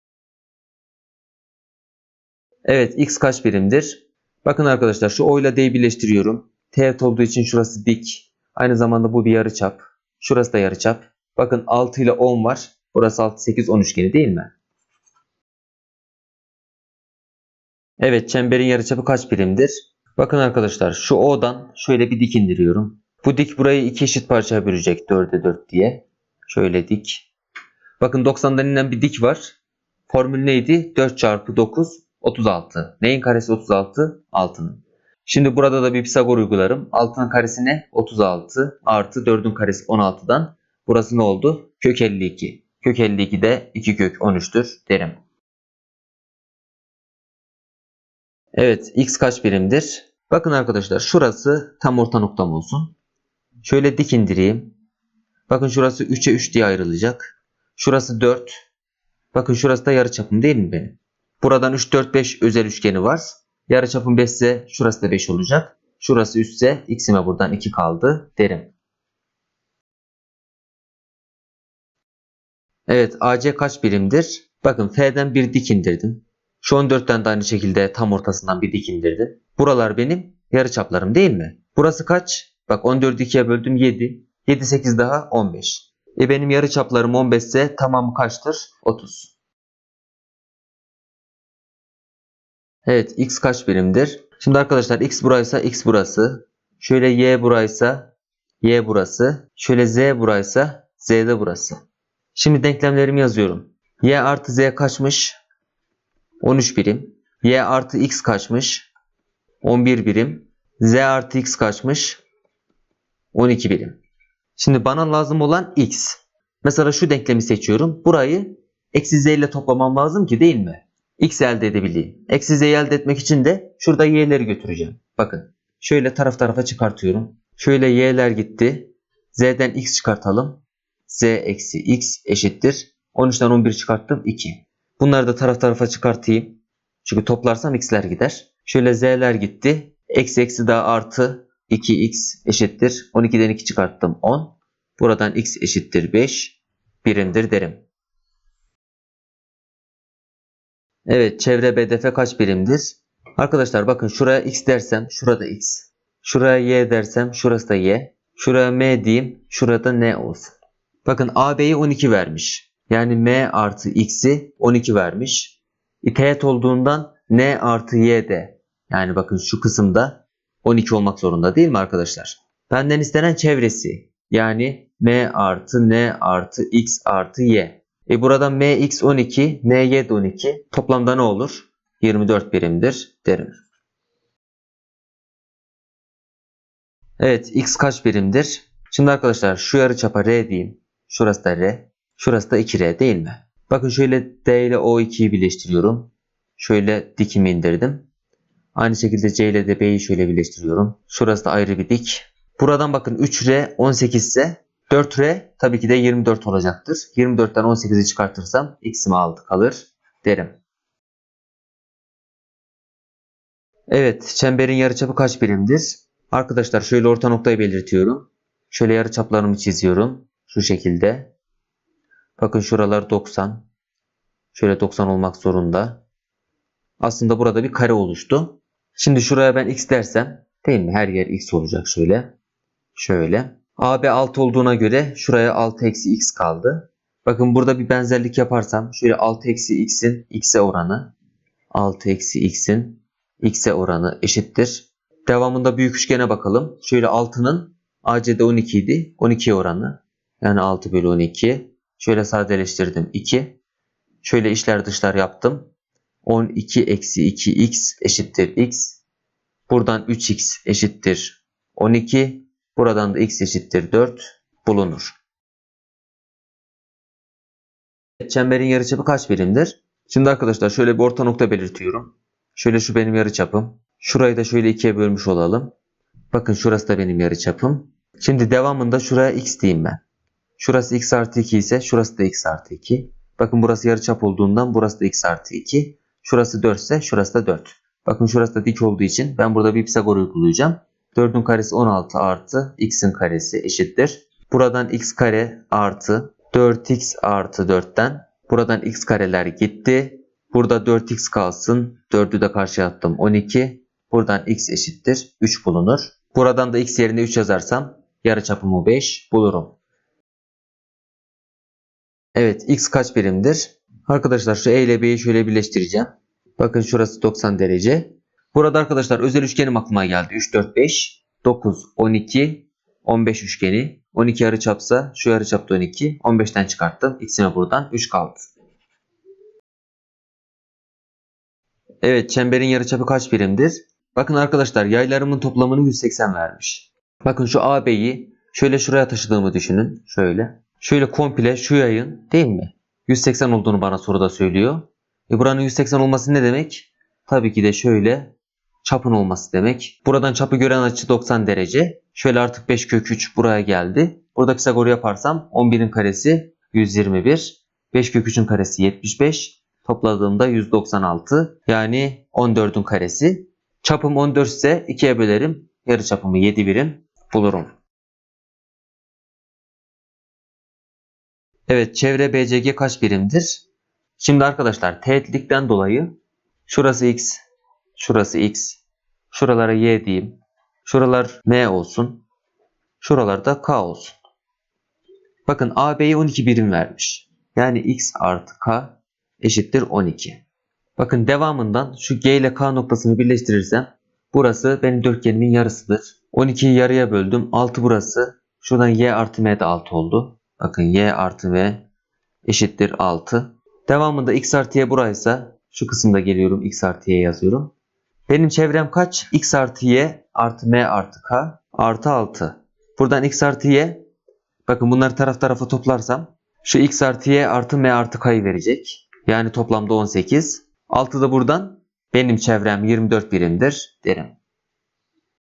TYT Geometri Soru Bankası Bölüm 19 Test 02 Soru Çözümleri
🎞 Soru Listesi
▶ 00:00:00 Bölüm
▶ 00:00:00 Soru 1
▶ 00:00:15 Soru 2
▶ 00:00:46 Soru 3
▶ 00:01:10 Soru 4
▶ 00:01:30 Soru 5
▶ 00:02:29 Soru 6
▶ 00:03:04 Soru 7
▶ 00:03:31 Soru 8
▶ 00:04:26 Soru 9
▶ 00:05:10 Soru 10
▶ 00:05:29 Soru 11
▶ 00:06:09 Soru 12